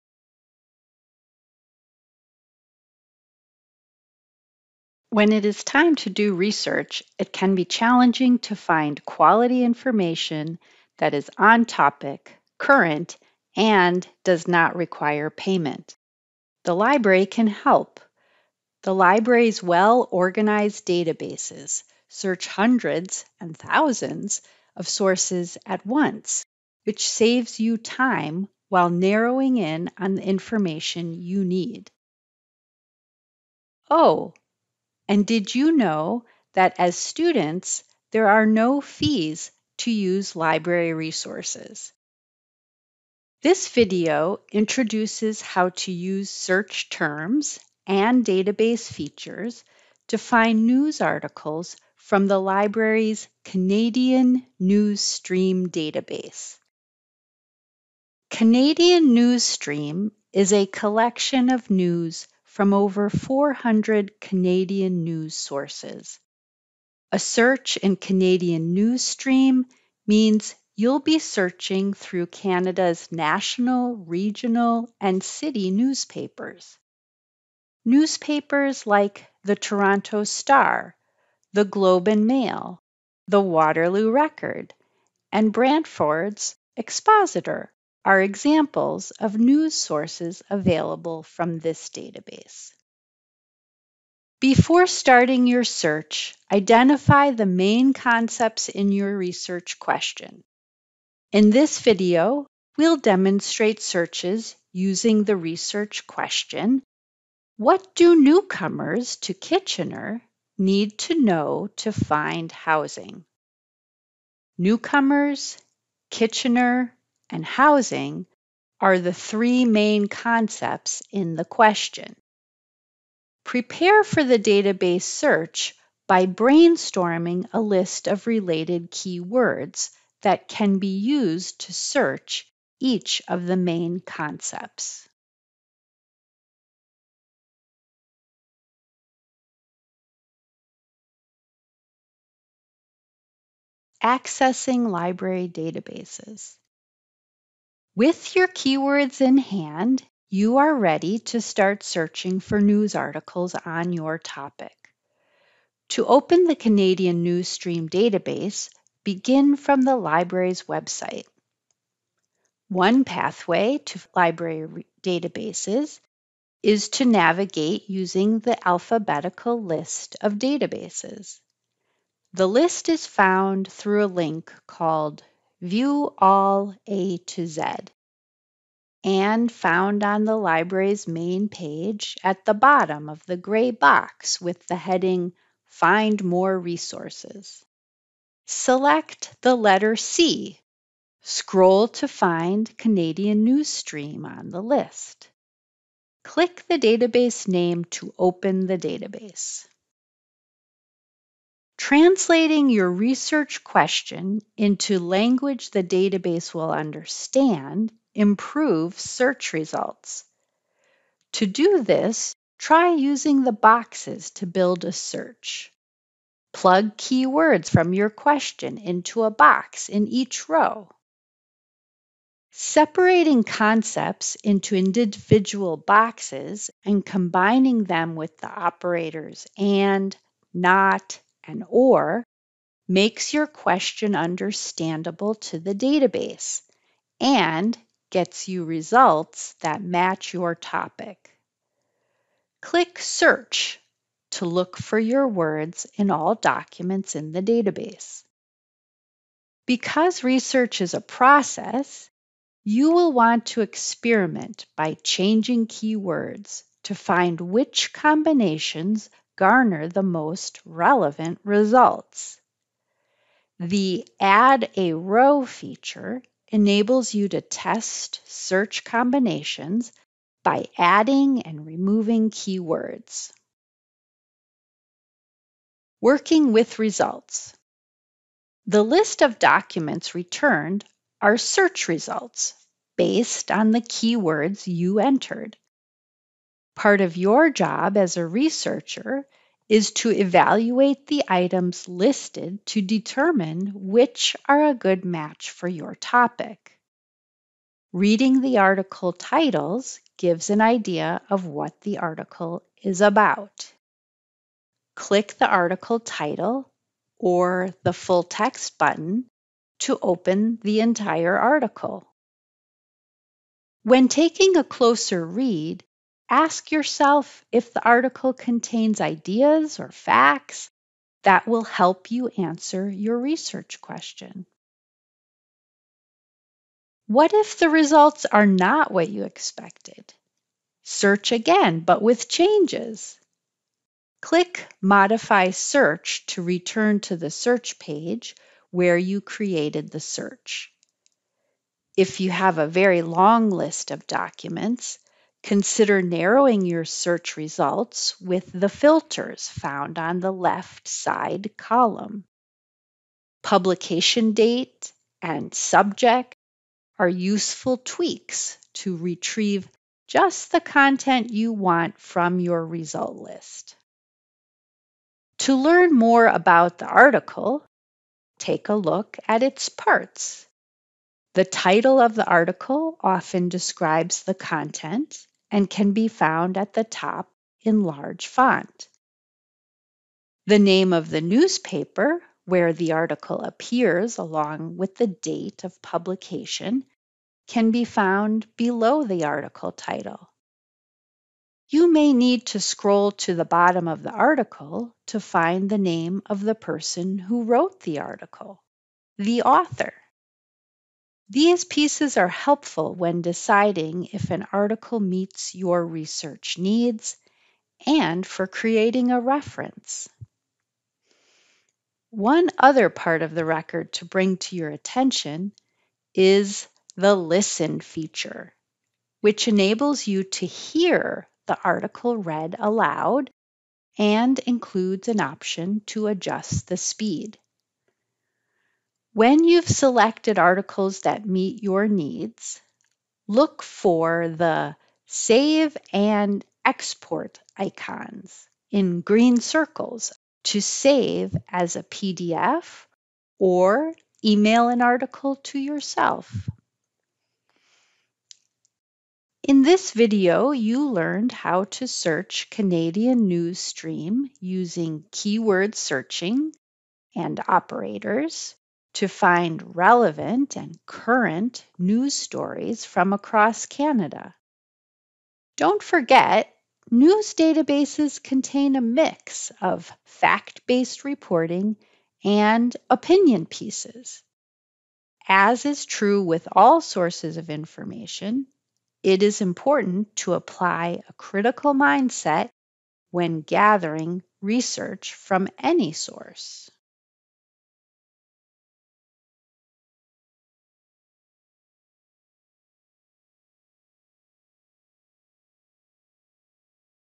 Finding News Articles - Canadian Newsstream
This video introduces how to use search terms and combine them with operators to find news articles from the Canadian Newsstream database.